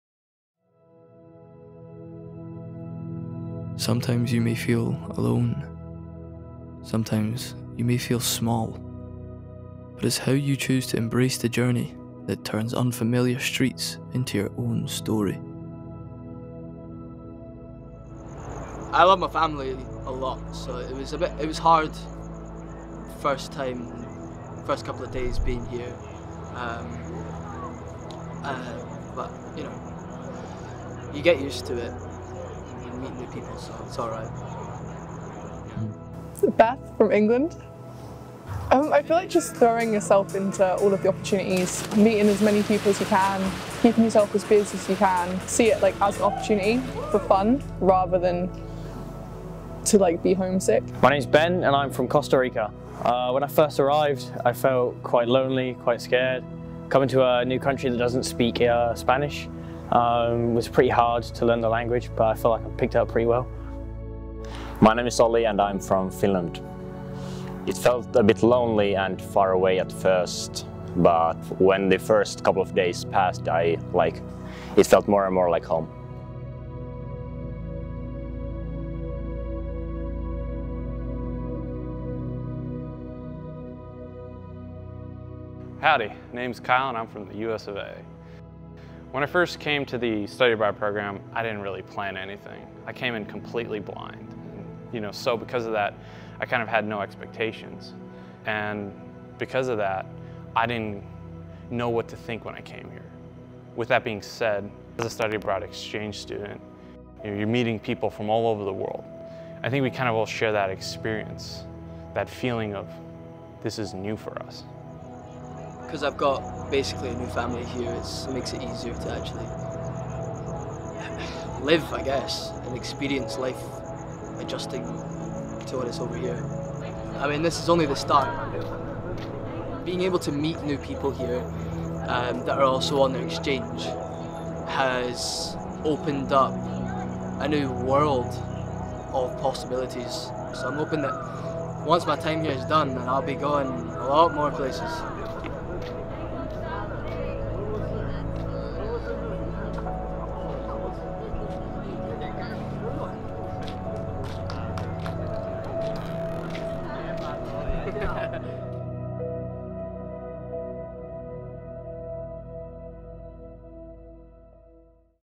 Deafening Silence - Short Documentary
Deafening Silence is a short documentary that was made to capture the feelings of students that are on their exchange and to share how they felt and then overcame this.